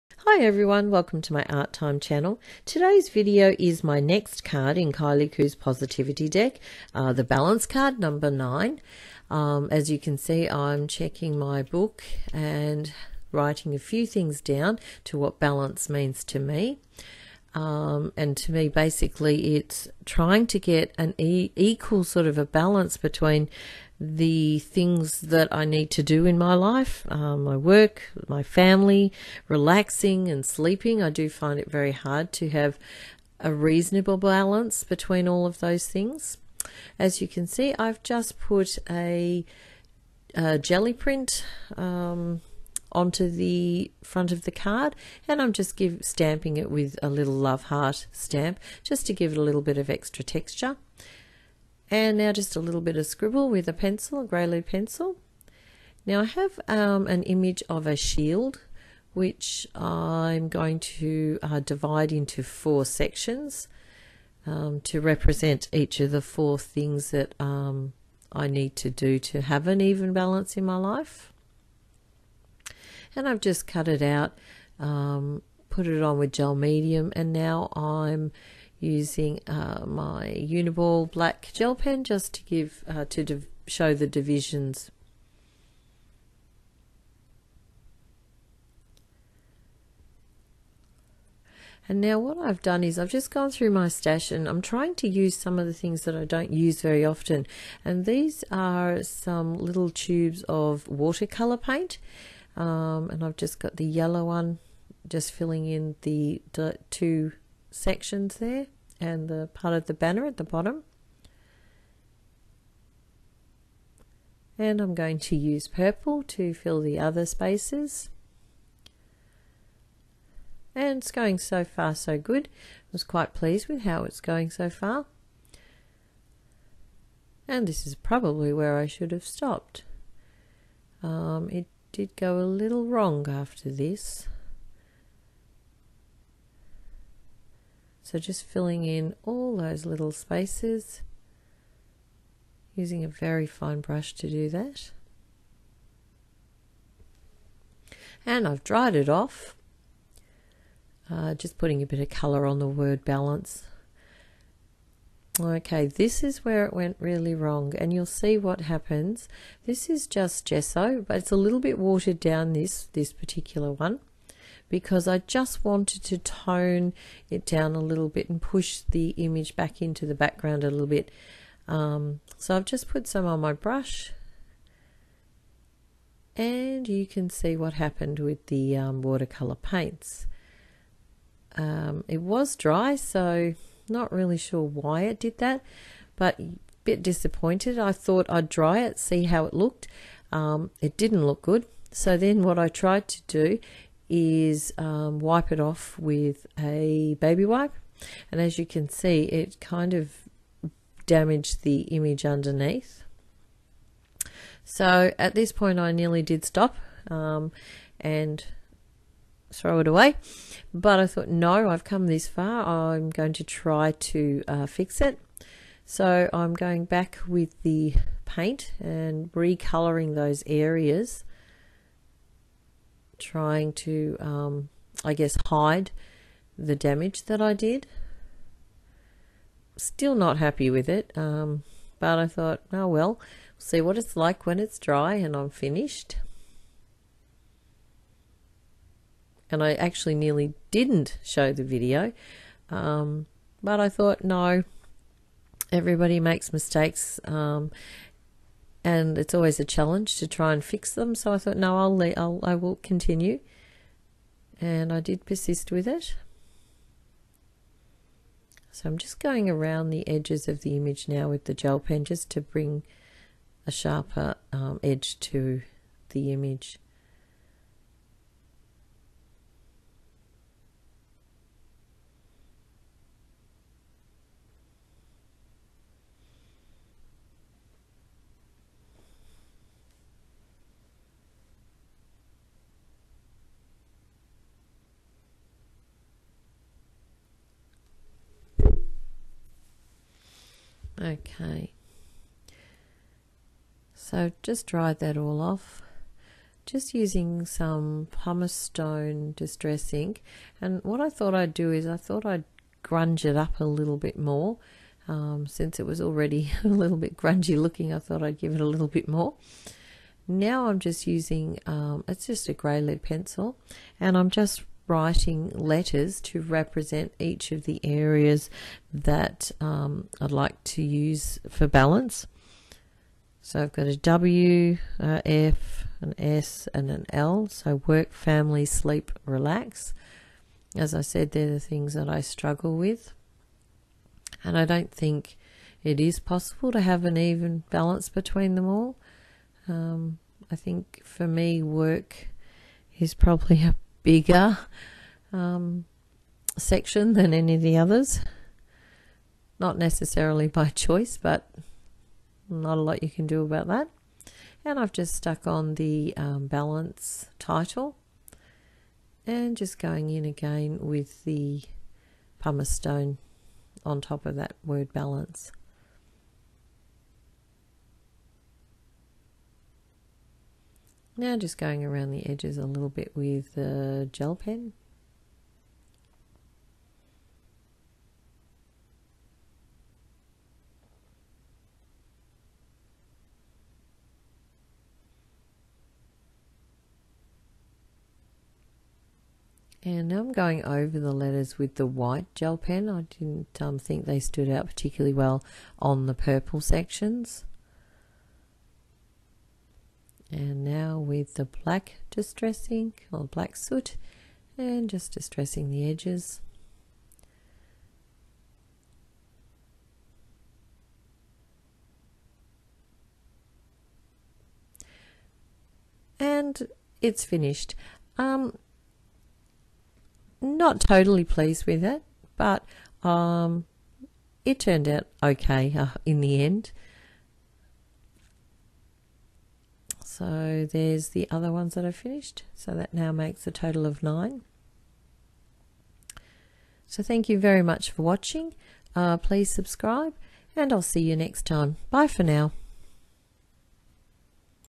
Balance
This ATC was made as part of a set for Kylie Coo's  Positivity deck.  I neearly a fail, I managed to salvage it (I think).

Art Craft Journals - Formally Art Time & Mixed Media Craft Time.